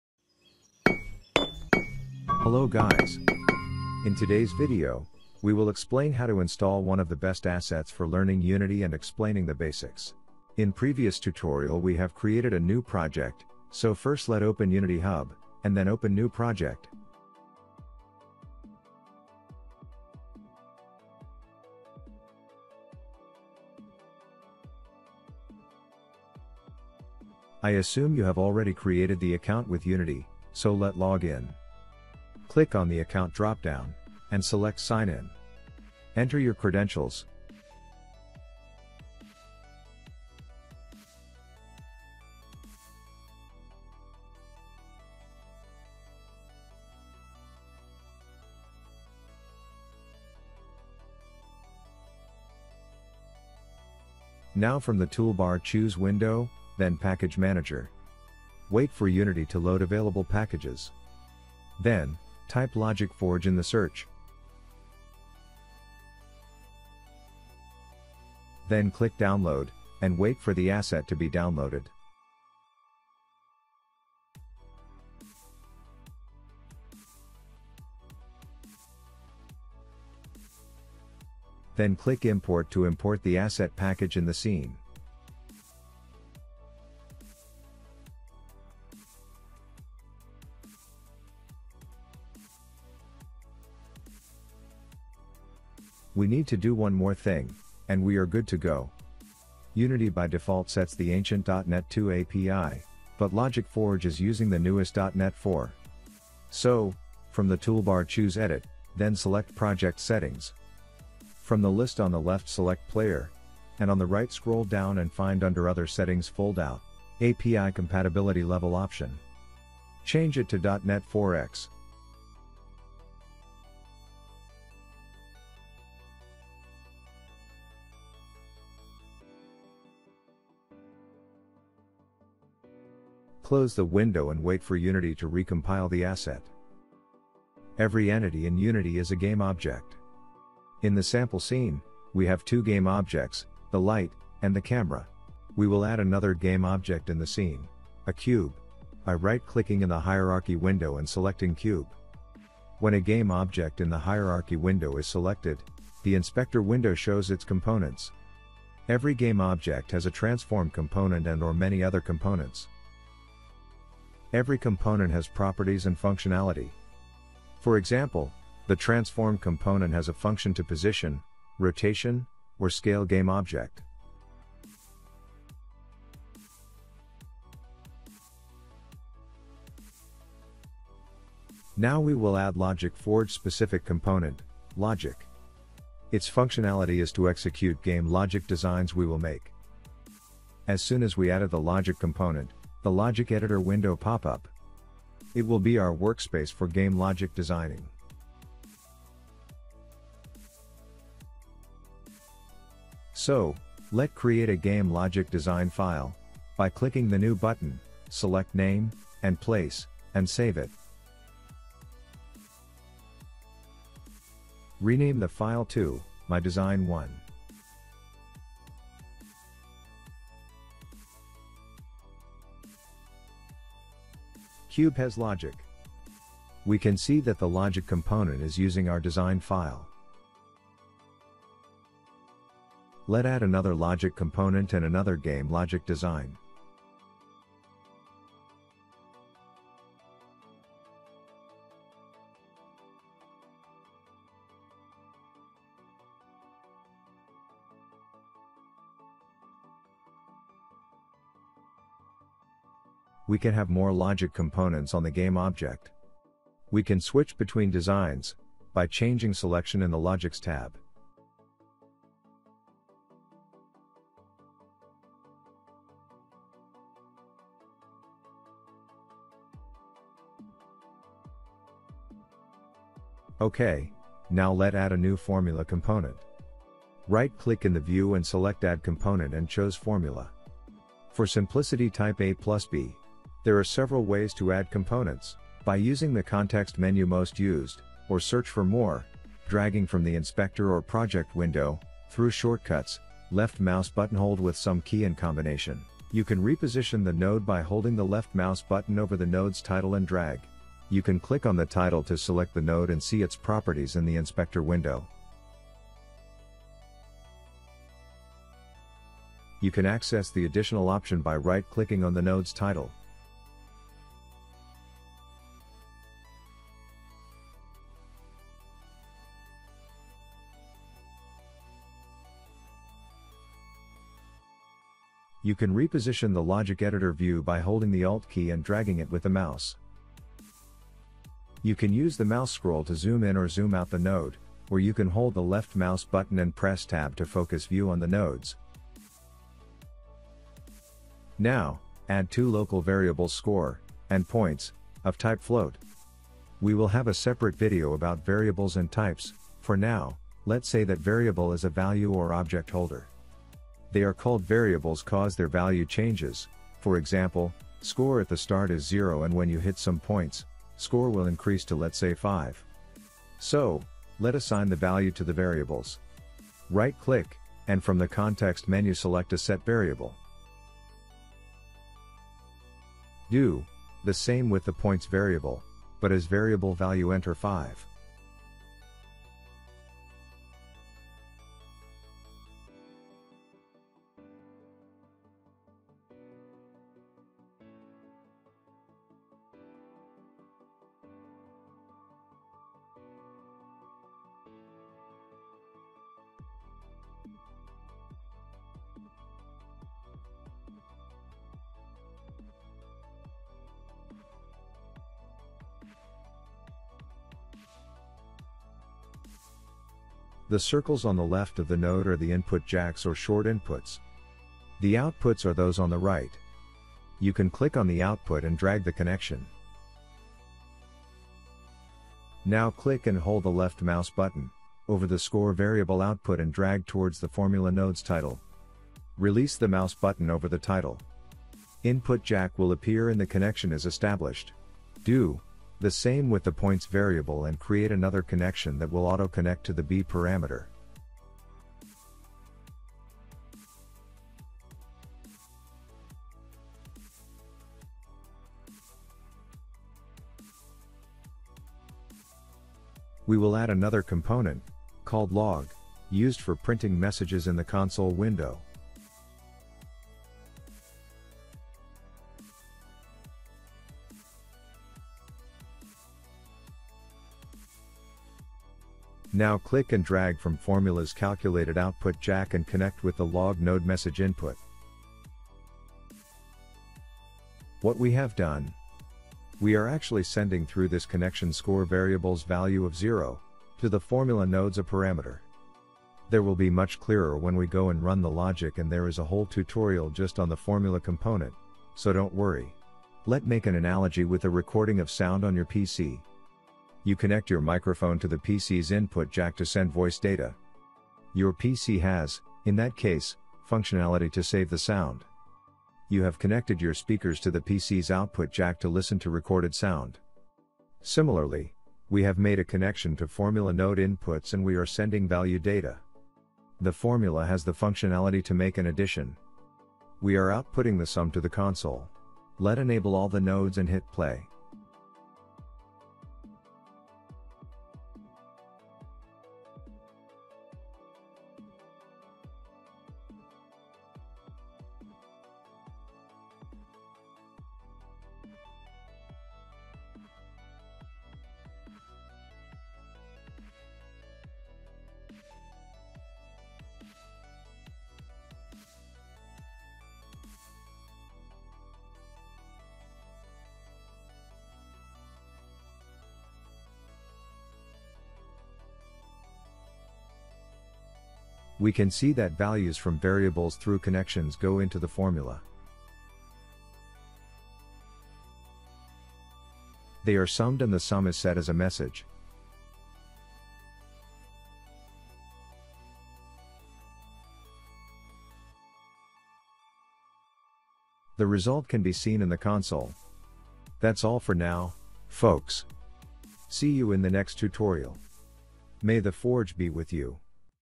Unity Visual Scripting - Hello Unity Hello Logic Forge part 2
In the beginning, there was a Forge. God has use this Forge to create Human Logic.
Unity Visual Scripting. Learn Input basics of visual scripting in Unity. by using visual scripting asset.  Create game logic and track the data flow in the visual scripting editor.